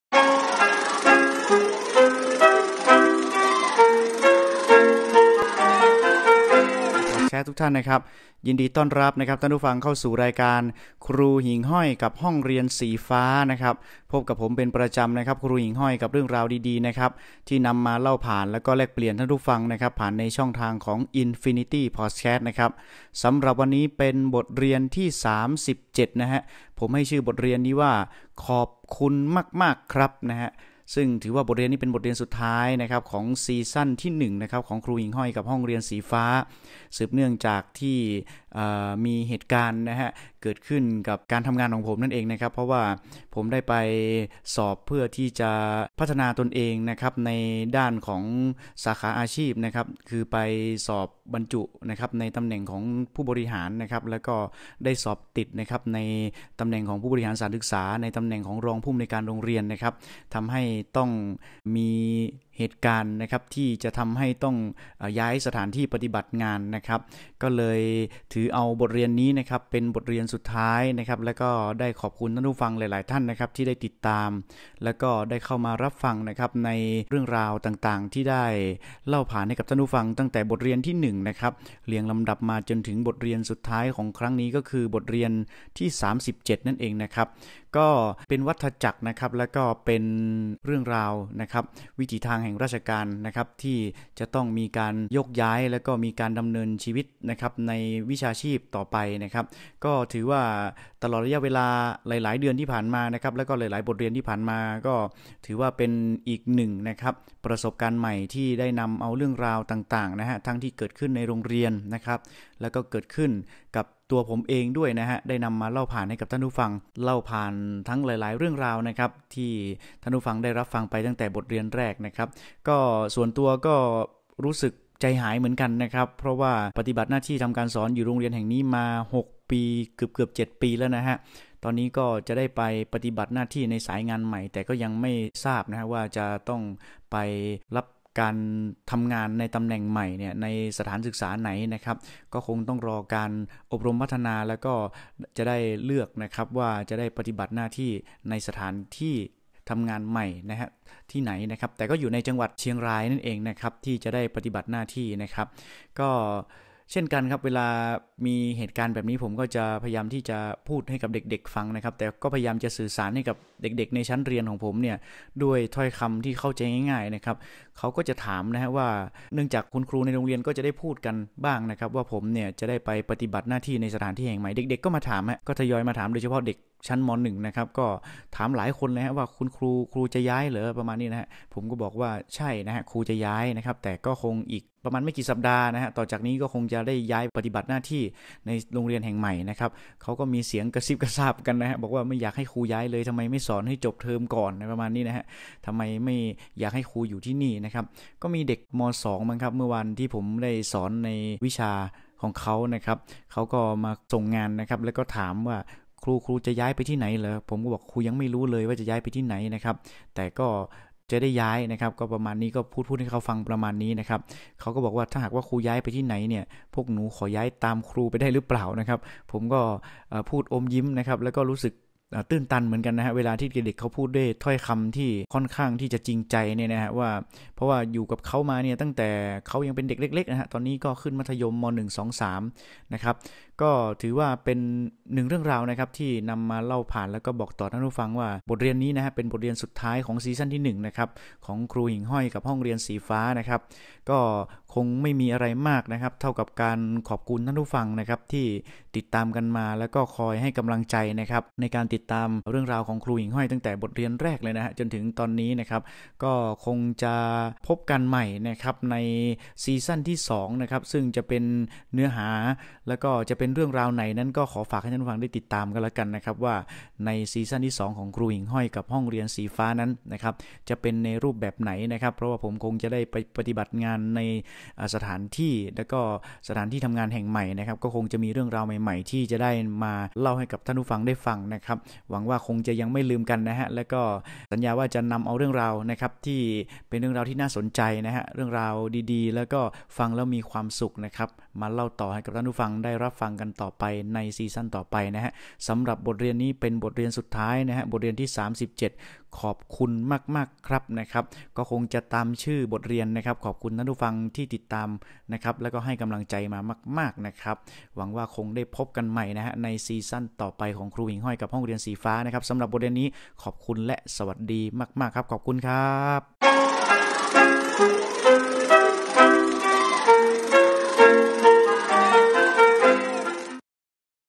ครูหิ่งห้อยกับห้องเรียนสีฟ้า 37: ขอบคุณมากๆครับ
สวัสดีมากๆครับคุณผู้ฟัง ผมครูหิ่งห้อยครับ สำหรับในอีพีนี้จะเป็นอีพีสุดท้ายของครูหิ่งห้อยกับห้องเรียนสีฟ้าใน Season ที่ 1 แล้วครับ ด้วยหน้าที่การงานของผมที่ต้องมีการดำเนินต่อไป ผมจึงจะไม่ได้สอนที่โรงเรียนนี้แล้ว
ขอบคุณคุณผู้ฟังที่ติดตามเรื่องราวของผมมาตลอด หวังว่าเราจะมาพบกับเรื่องราวใหม่ๆใน Season 2 นะครับ :)
ขอบคุณมากๆครับ